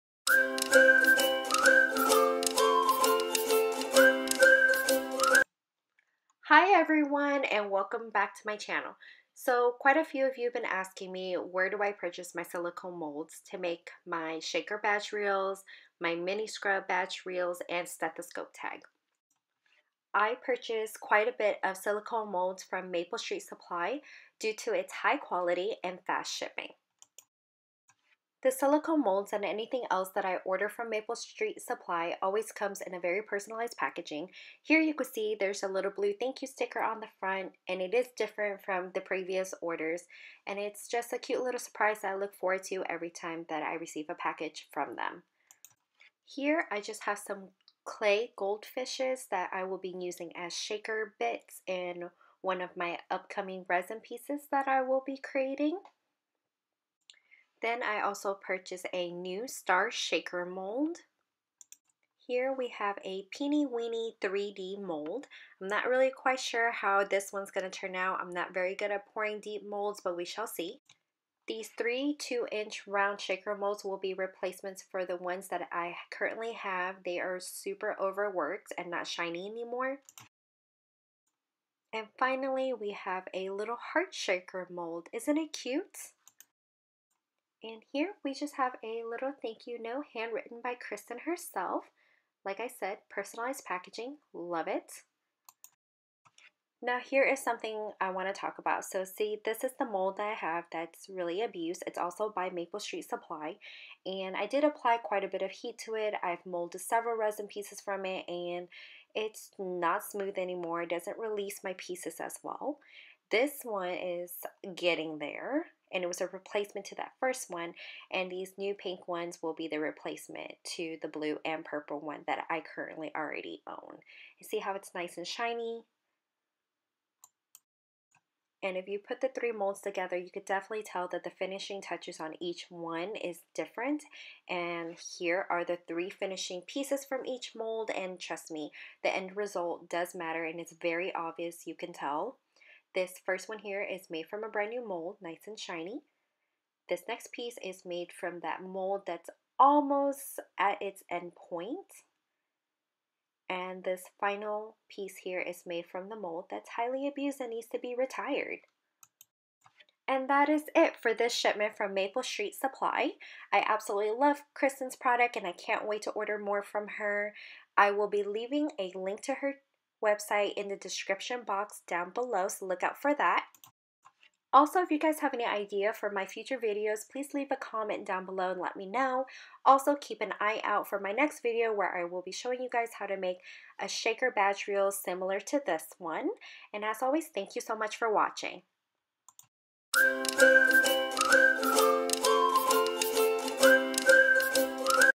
Where To Buy Silicone Molds
A quick haul on silicone molds! Also, a little info regarding on the outcome of resin pieces from different finishes of silicone molds.

Promarine Epoxy
Silicone Resin Tools
Raised Silicone Mat
E600 Glue
Transfer Tape
Neenah Cardstock
HP Printer
Cricut Mats
Badge Reels
Cellophone Bags @CutePupCrafts